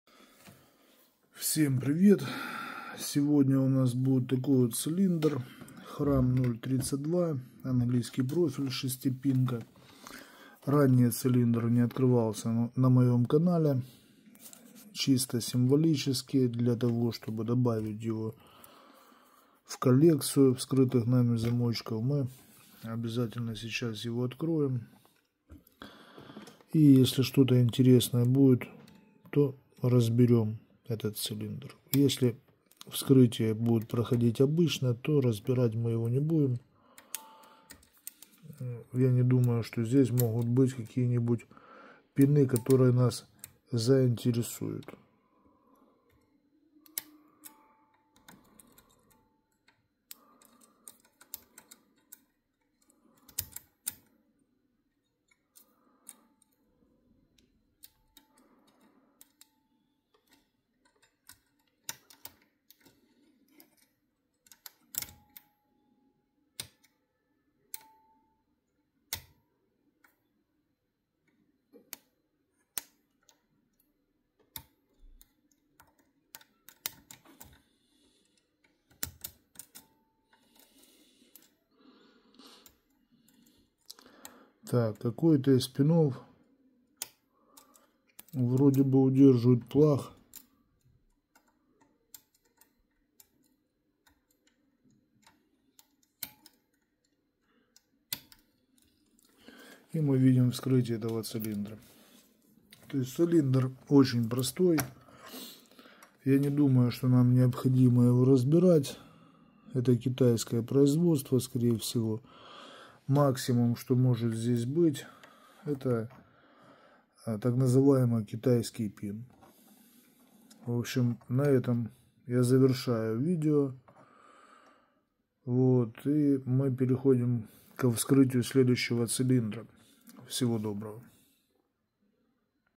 Вскрытие цилиндра hram032
В этом видео вы увидите вскрытие цилиндра hram032. Это бюджетный китайский цилиндр, который ранее на моем канале не открывался. Сколько по времени потребуется на вскрытие этого цилиндра, смотрите в этом видео.
Если вы хотите обучиться вскрытию замков без повреждения, пишите в телеграмм @sv040.